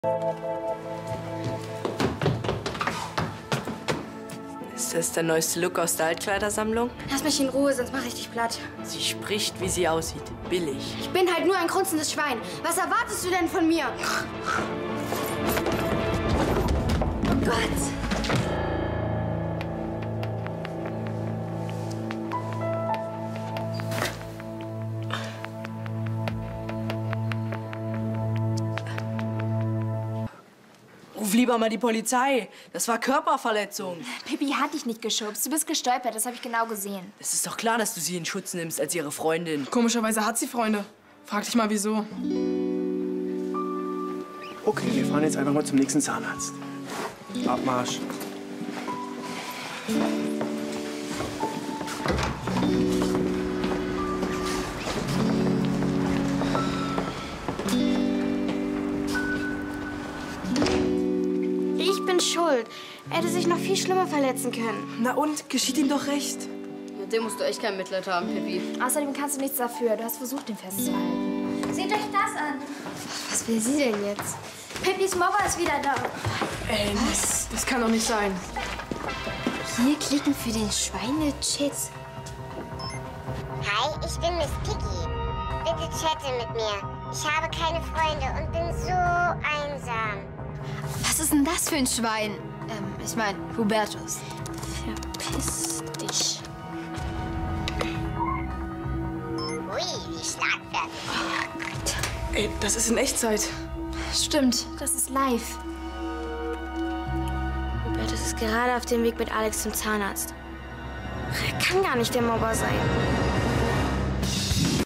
Pippi stellt Hubertus zur Rede
Pippi wird im Unterricht mit anonymen Beleidigungen konfrontiert. Von „Schweinchen Piggy" ist die Rede. Pippi versucht Haltung zu bewahren, doch die Aussagen treffen sie schwer. Sie vermutet Hubertus hinter der Aktion. In der Zwischenzeit erscheinen im Internet weitere beleidigende Animationen. Tobias und Jonny stellen Hubertus zur Rede. Der weist aber jede Schuld von sich. Pippi ist verzweifelt und als erneute Aktionen öffentlich werden, verliert sie ihre Fassung und brüllt Hubertus an. Erschrocken stolpert er und stürzt die Treppe hinunter.